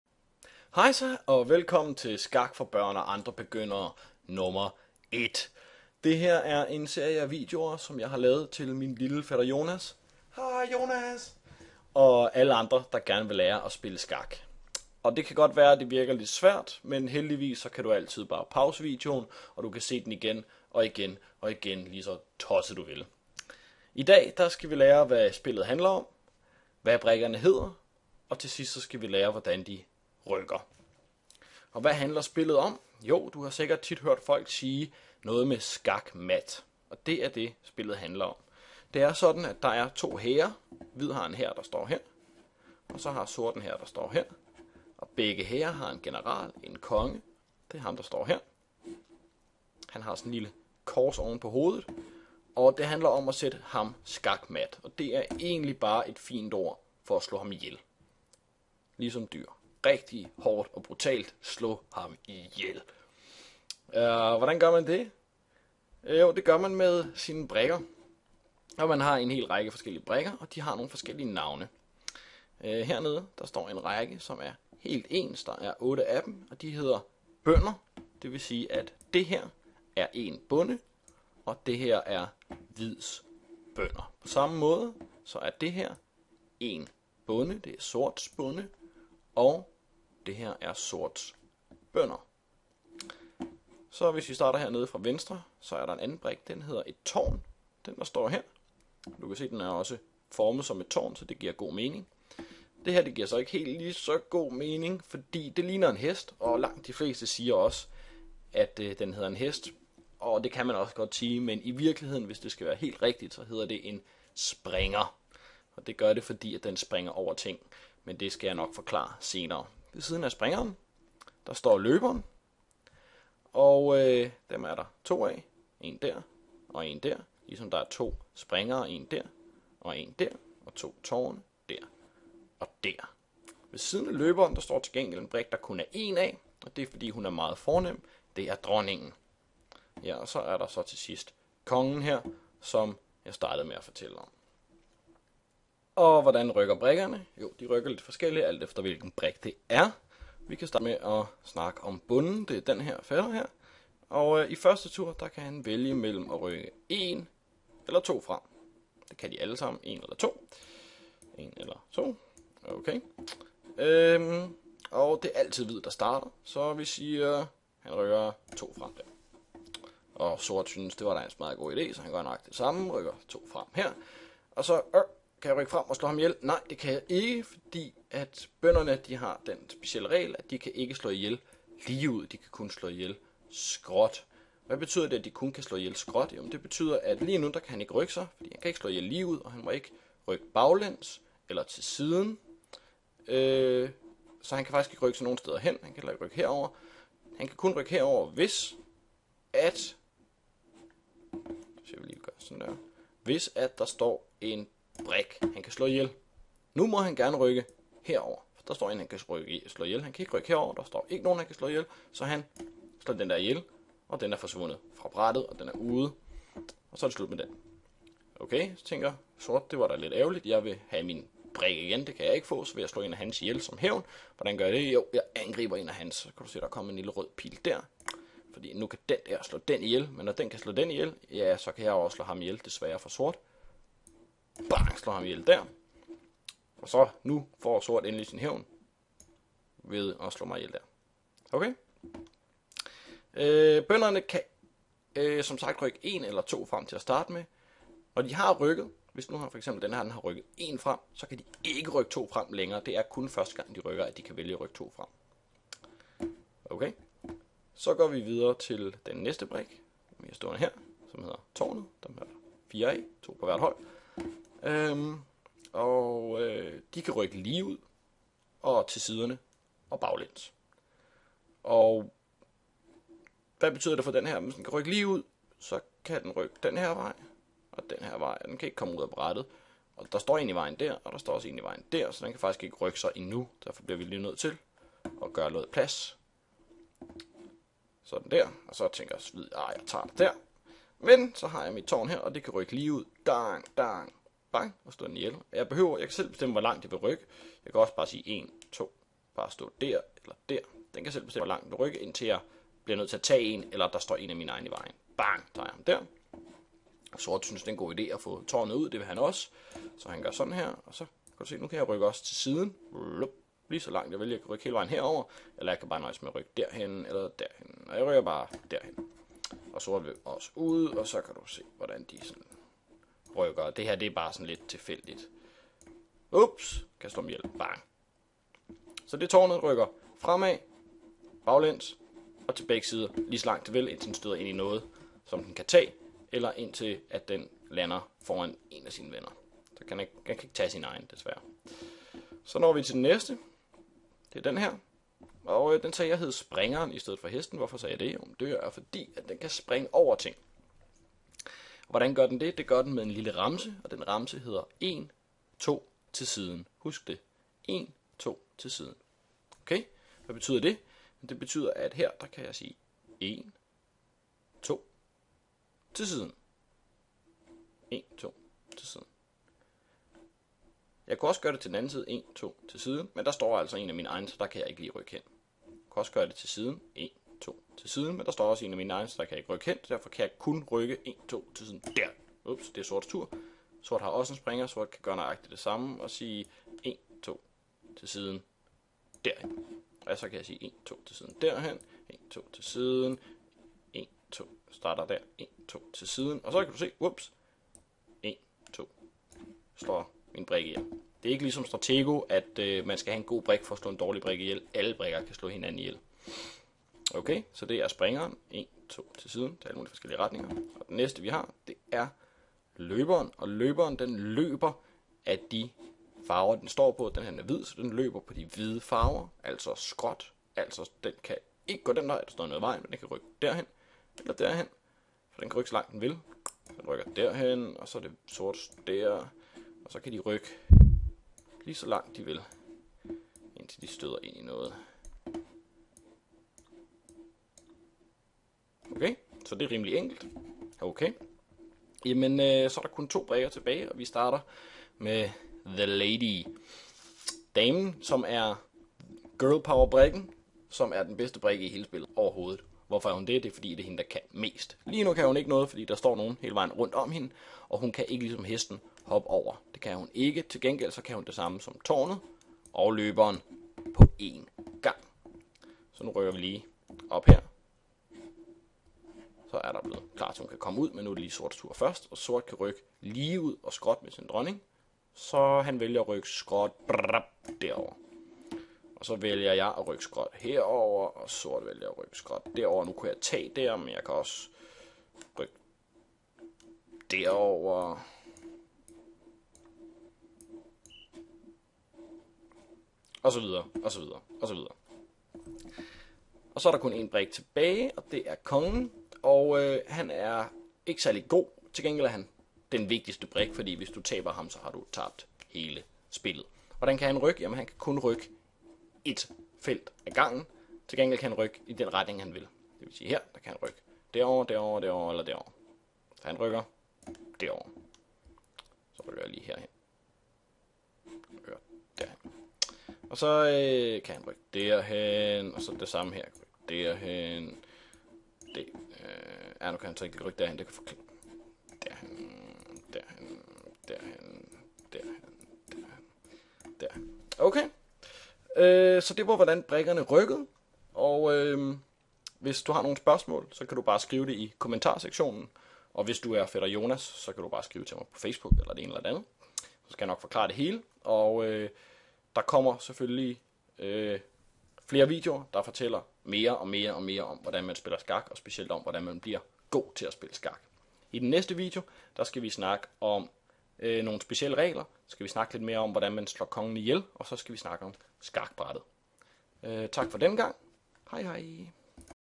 Skak for børn og andre begyndere 1: Hvordan brikkerne rykker
"Skak for børn og andre begyndere" er en serie af videoer der kan lære dig alt hvad der er værd at vide om skak. Vi starter heeeelt nede fra bunden med de allermest basale ting og så arbejder vi os ellers hele vejen op til de finere aspekter af det smukke spil. God fornøjelse!